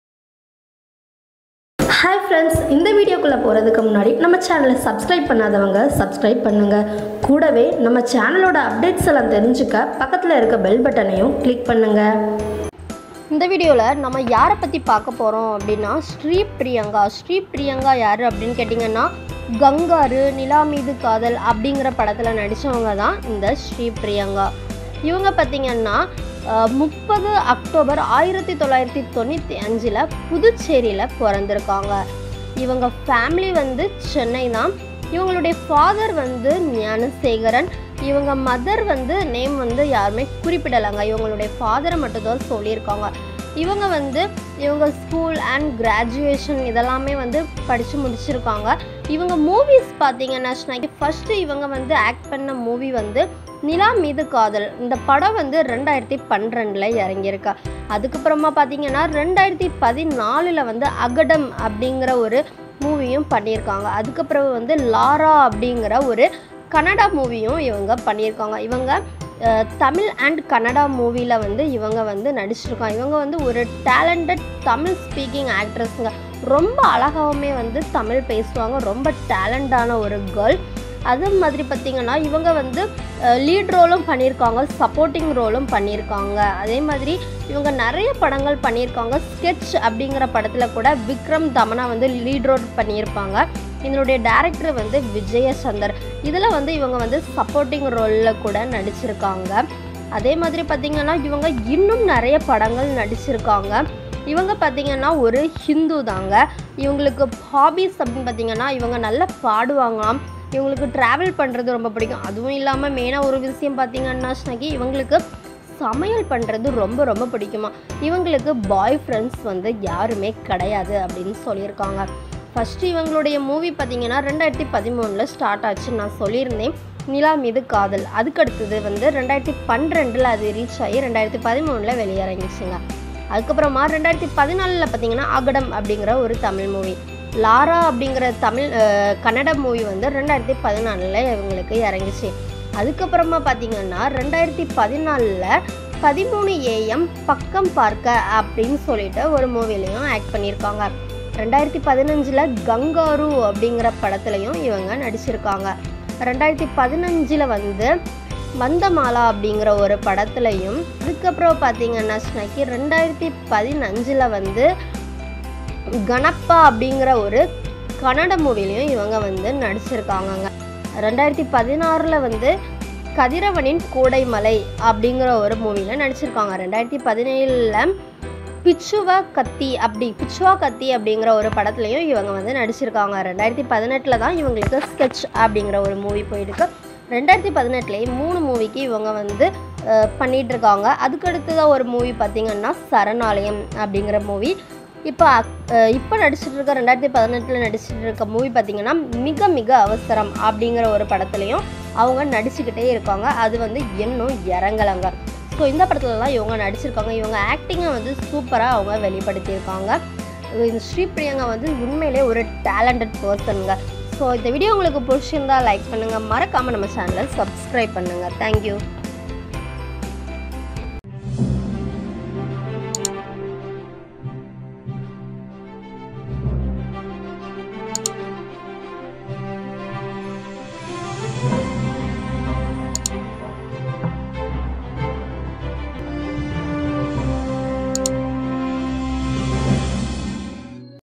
Sketch Movie Actress Sri Priyanka Interesting Biography | யார் இந்த ஸ்ரீ பிரியங்கா?
This video is about the interesting biography of Sketch movie Actress Sri Priyanka who is friend of Tamannah in sketch movie with vikram